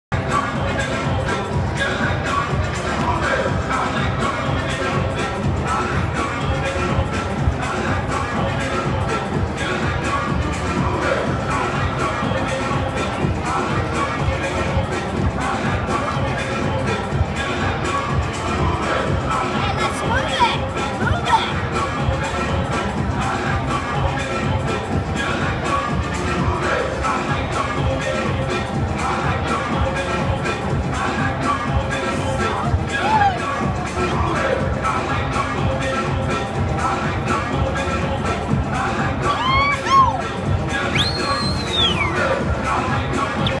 Dancing with the Arizona Diamondbacks
When our family's travels took us to Phoenix, Arizona, a baseball game was a must. Our kids loved getting down to the music at Chase Field at a spring Dbacks game.